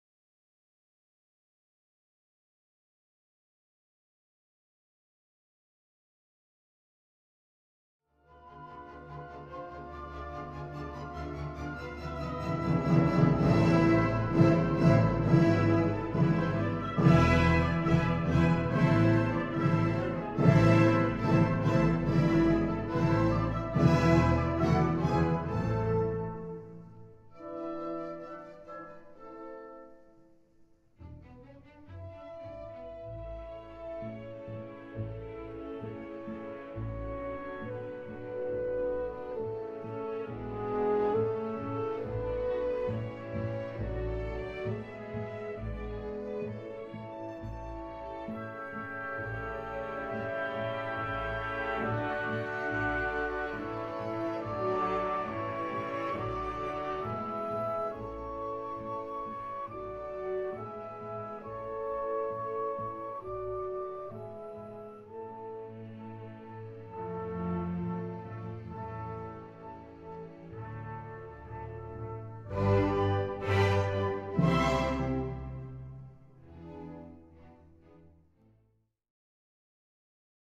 OSM Polyphonique Walk Thru Music
A demo WalkThruMusic  experience of the OSM Polyphonique Application,  captured in the first person by a user navigating the Montréal Symphony Orchestra virtual concert hall.

Application developed by Zack Settel/6Dmusic.